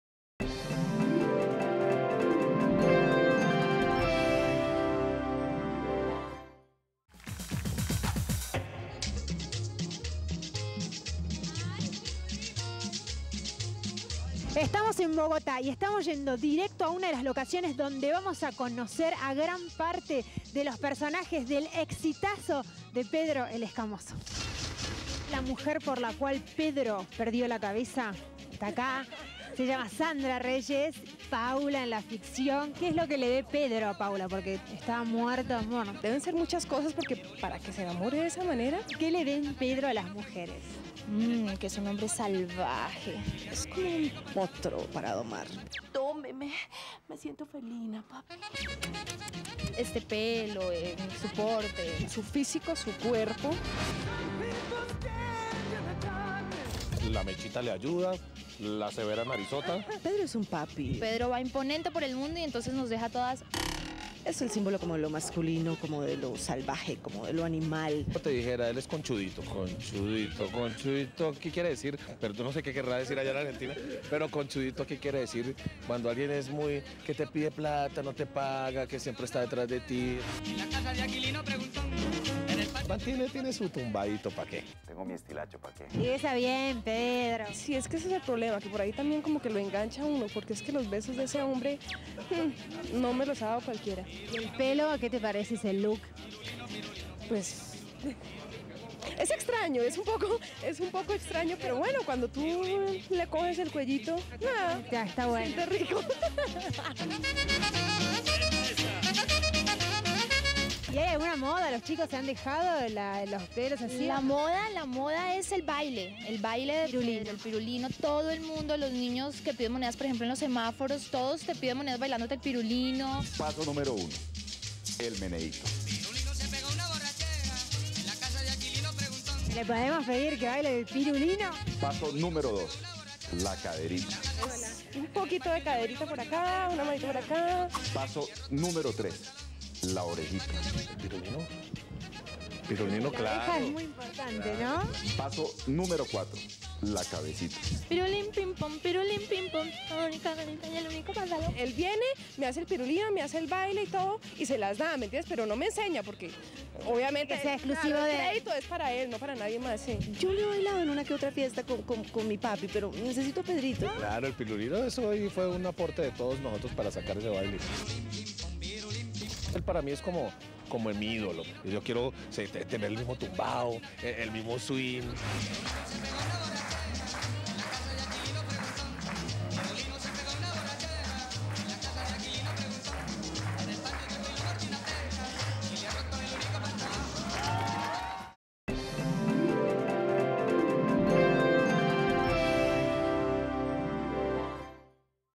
Nota a los actores de Pedro el Escamoso - Versus 2001
A continuación no te podes perder una entrevista increíble.
Ahora: Nota a los actores de Pedro el Escamoso